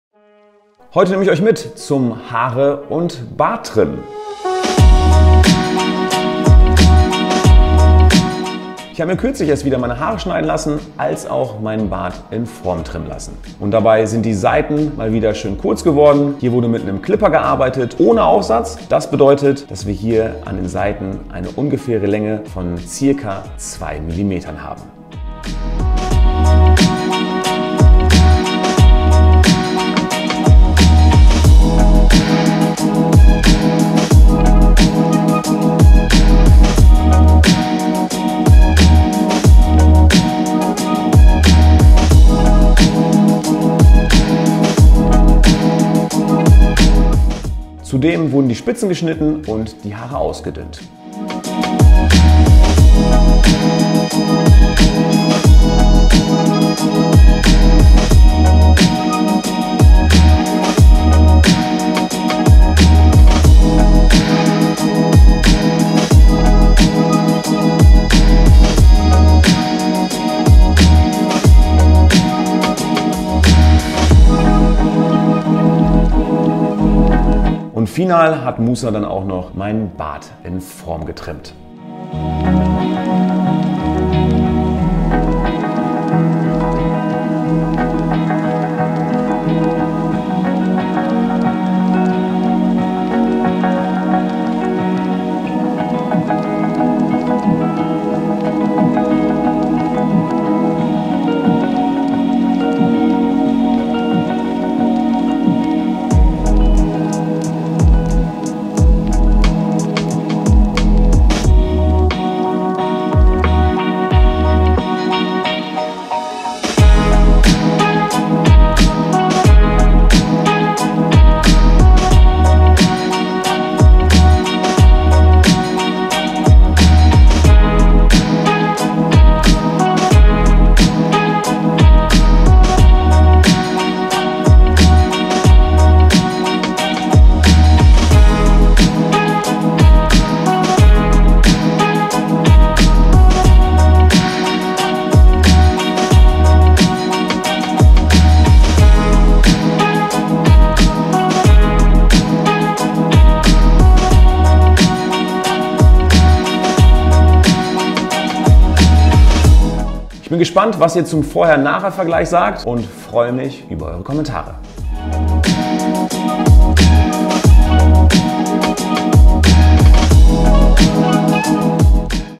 NEW HAIRCUT and BEARD TRIM ● HAARSTYLING VIDEO
Hey Leute
nach einiger Zeit war ich mal wieder bei einem Barber, welcher mir meine Haare und meinen Bart wieder auf Vordermann gebracht hat. Der letzte Friseurbesuch lag schon eine gute Zeit zurück und deshalb wurde es mal wieder Zeit meine Haare und meinen Bart passend zu schneiden und zu trimmen. In diesem Video seht ihr also, wie meine Haare geschnitten werden und wie mein Bart wieder auf Vordermann gebracht wird. Schreibt gerne in die Kommentare, wie euch der Haarschnitt gefallen hat und schreibt auch gerne in die Kommentare, falls ihr weitere Videos dieser Art sehen möchtet. :)